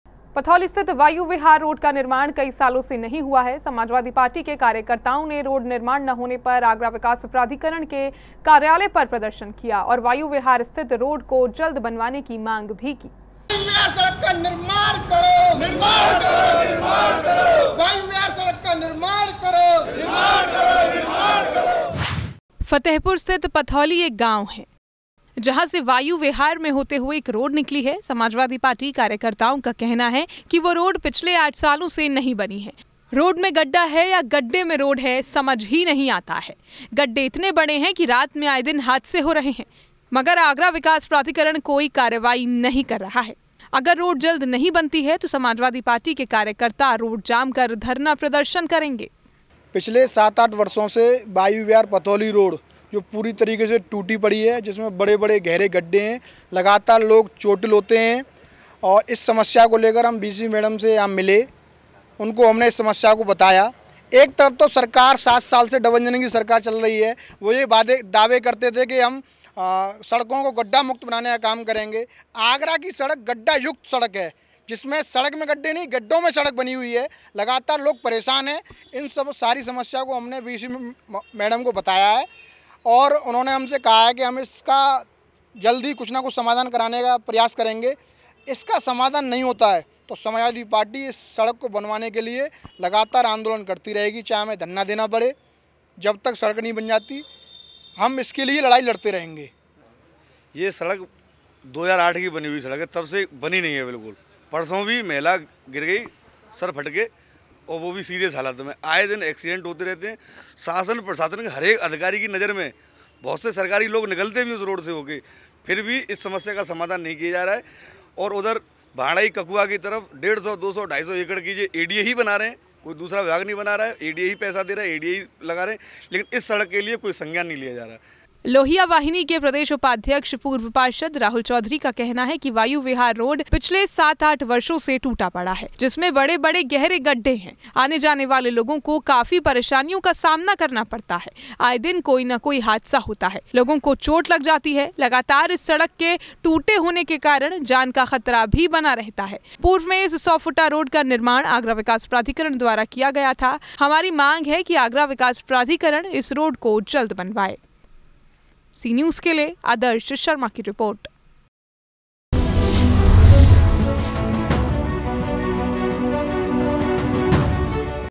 Patholi : सपा का एडीए दफ्तर में प्रदर्शन | Samajwadi Party workers |Development Authority |Sea News
पथोली स्थित वायु विहार रोड का निर्माण कई सालो से नहीं हुआ है, समाजवादी पार्टी के कार्यकतार्ओं ने रोड निर्माण न होने पर आगरा विकास प्राधिकरण के कार्यालय पर प्रदर्शन किया, और वायु विहार स्थित रोड को जल्द बनवाने की माँग की